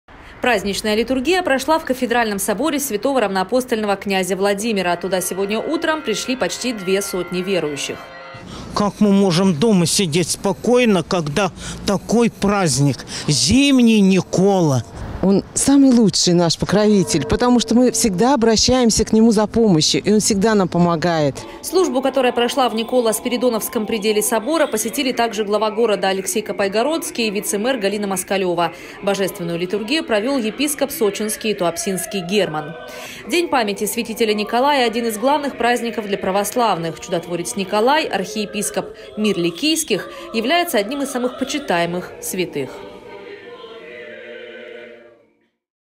В сочинских храмах сегодня отметили день Николая Чудотворца. Новости Эфкате
Праздничная литургия прошла в Кафедральном соборе святого равноапостольного князя Владимира. Туда сегодня утром пришли почти две сотни верующих.

Службу, которая прошла в Николо-Спиридоновском приделе собора, посетили также глава города Алексей Копайгородский и вице-мэр Галина Москалёва. Божественную литургию провел епископ Сочинский и Туапсинский Герман.

День памяти святителя Николая — один из главных праздников для православных. Чудотворец Николай, архиепископ Мир Ликийских, является одним из самых почитаемых святых.